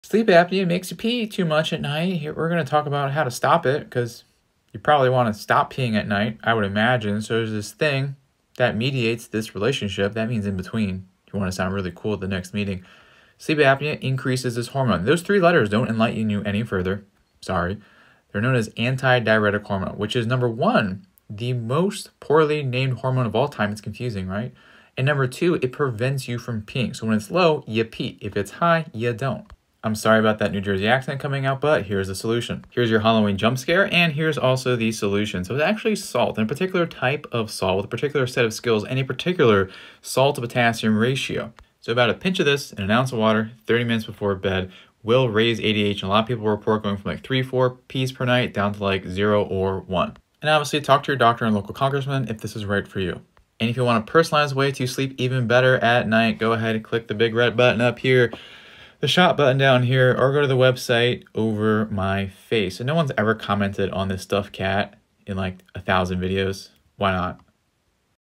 Sleep apnea makes you pee too much at night — here’s how to stop it
(Tap @apneareset to get your clear, personalized path to better sleep)

If you’re waking up multiple times a night to pee, it’s not always your bladder’s fault.
Sleep apnea itself can cause that by lowering a key hormone called **ADH** — anti-diuretic hormone.

ADH’s job is to *prevent* you from peeing at night.
But when apnea interrupts your breathing, it also disrupts your brain’s release of ADH.
Low ADH = more nighttime trips to the bathroom.

Here’s the quick fix: restoring your body’s natural ADH rhythm with better electrolyte balance.
A small pinch of salt (balanced with potassium) in an ounce of water about 30 minutes before bed can help raise ADH levels and reduce nighttime urination.
Many people see their bathroom visits drop from 3–4 times a night to just once — or none.

That’s a helpful trick — but the real results come when your plan matches your body.